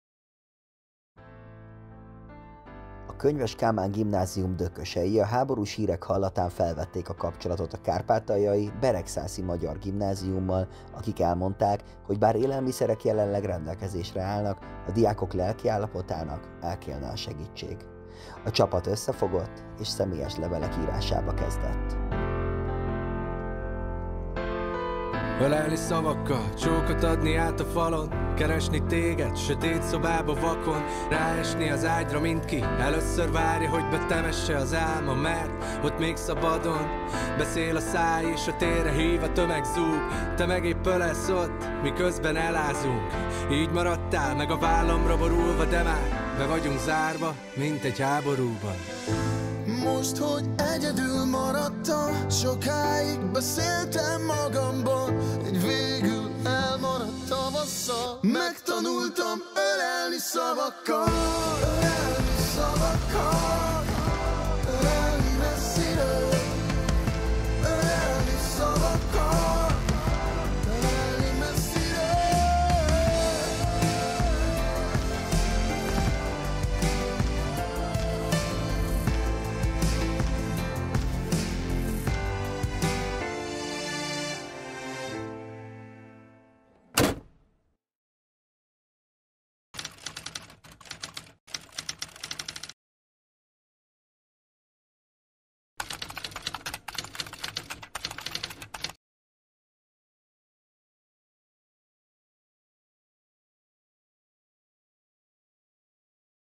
Határokon átívelő segítség a Könyvesben
A Könyveses Dökösök leveleket írtak a Beregszászi Bethlen Gábor Líceum minden egyes diákjának. Elképesztő összefogásnak köszönhetően értek célba a diákjaink megható gondolatait tartalmazó levelek egy kis ajándékcsomag kíséretében.

A videót készítette: Hartmann Levente